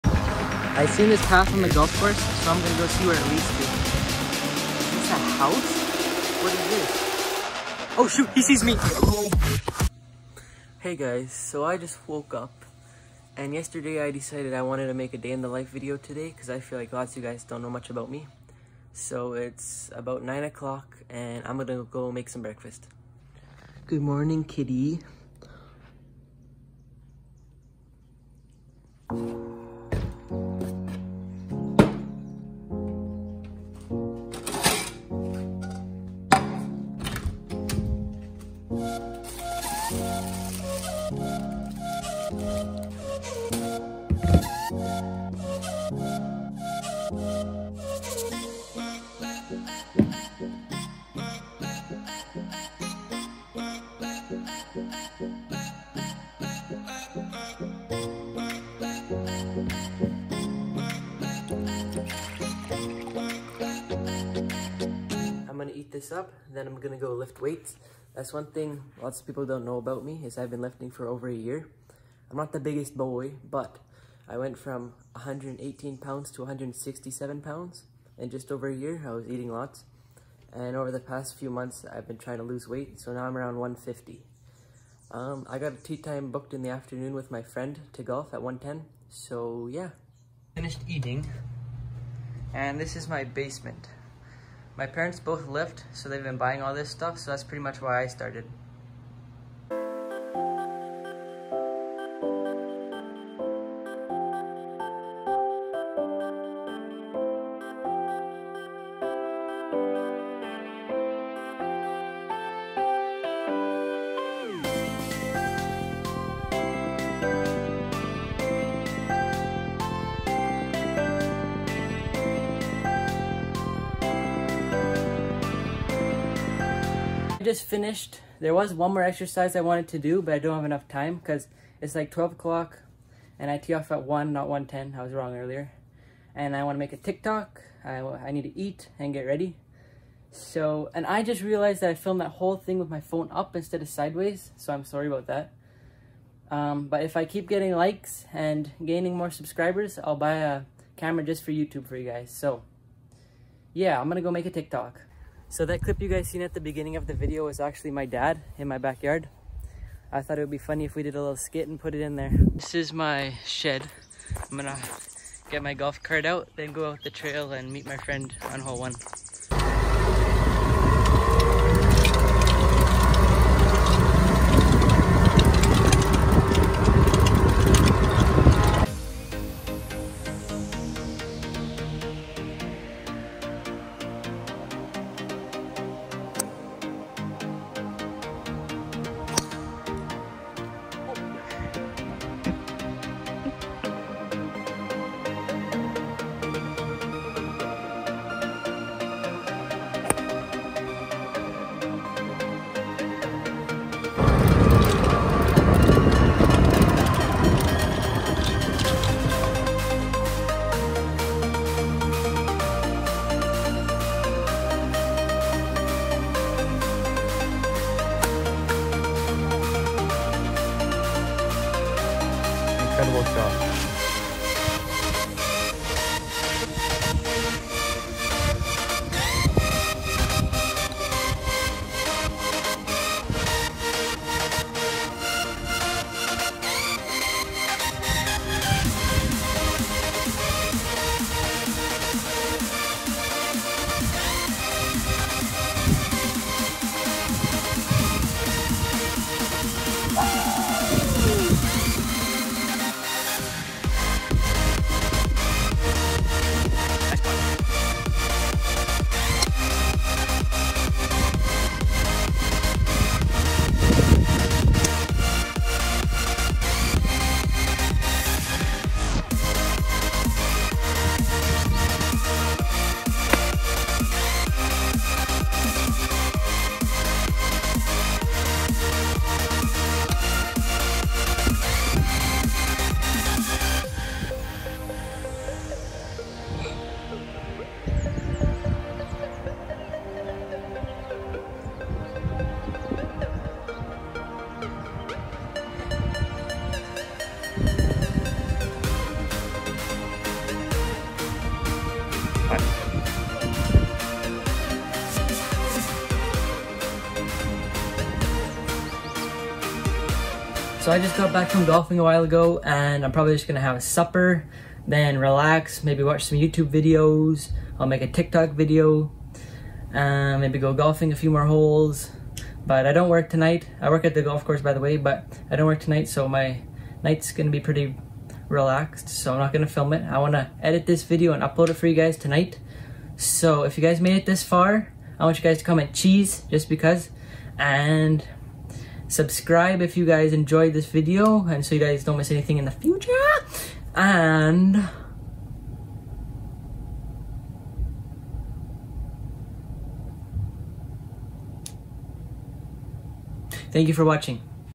A DAY IN THE LIFE OF A TIKTOKER! | Luke Davidson
In this video I show you guys what i do on a regular day, most of you don't know much about me from my TikToks.

DONT FORGET TO GIVE THE VIDEO A THUMBS UP IF YOU ENJOYED!

LET'S TRY AND GET TO 25,000 BY SEPTEMBER!